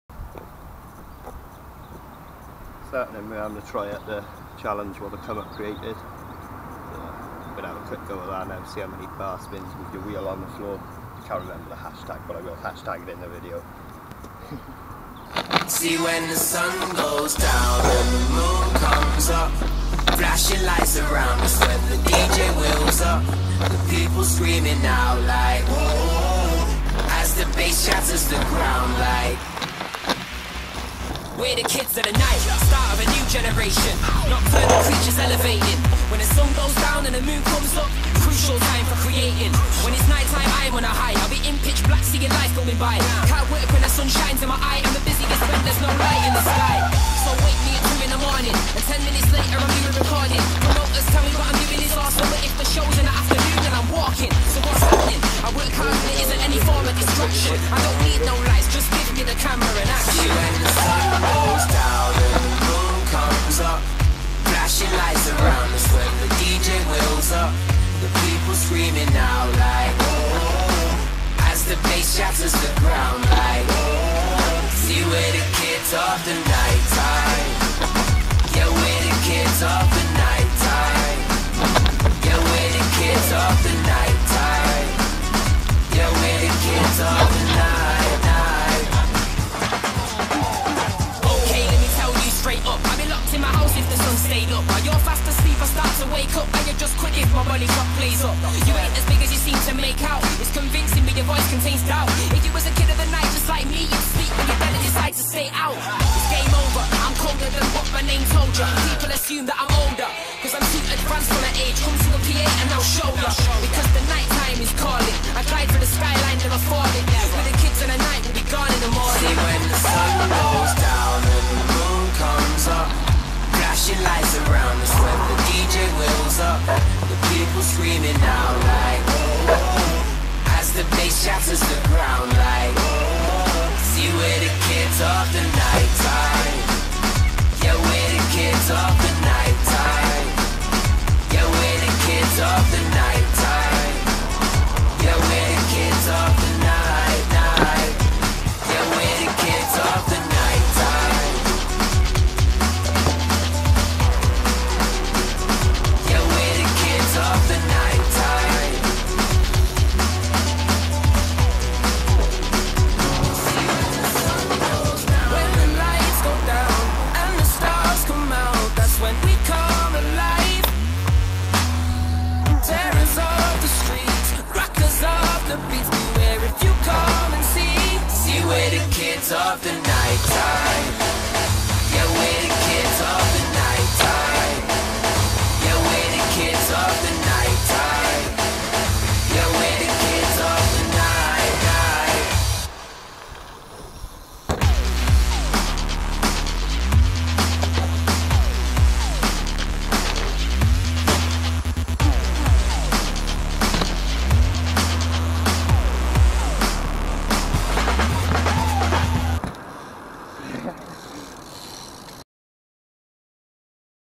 The Come Up BMX Challenge
Had A Go At The radchallenge And Then Had A Quick Sesh Put Into A Music Edit.

Much Love